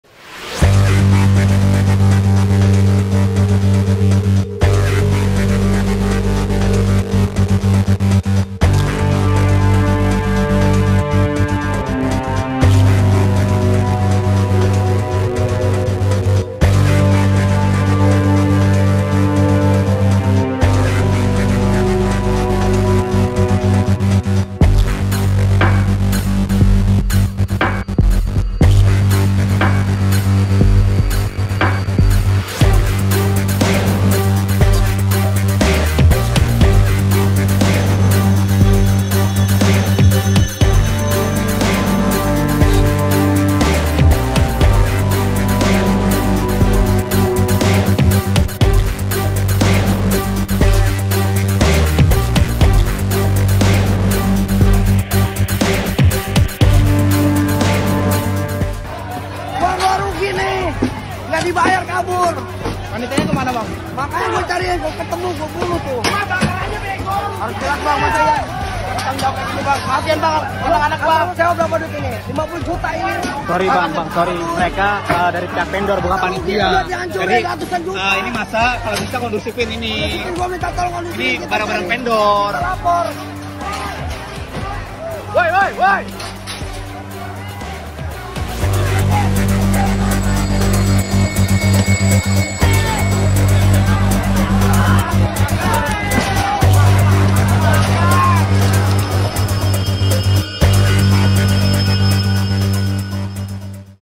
Ketua Panitia Konser Lentera Festival Bawa Kabur Uang Ratusan Juta, Kini Diburu Polisi
Ketua Panitia Konser Lentera Festival, Muhammad Dian Permana kini menjadi buruan polisi usai diduga menggelapkan uang hingga ratusan juta dan membuat acara menjadi kacau.
Kapolsek Pasar Kemis Ucu Nuryandi menegaskan kini pihaknya telah memeriksa 5 orang saksi dari pihak event organizer dan 3 orang saksi dari panitia penyelenggara.
Panitia konser mengaku bahwa mereka mengalami kerugian hingga Rp800 juta karena uang dibawa kabur oleh Dian.
Akibat konser yang terbengkalai, para penonton yang kecewa pun melampiaskan amarahnya pada vendor yang bertugas. Atas hal itu, vendor pun mengalami kerugian hingga ratusan juta.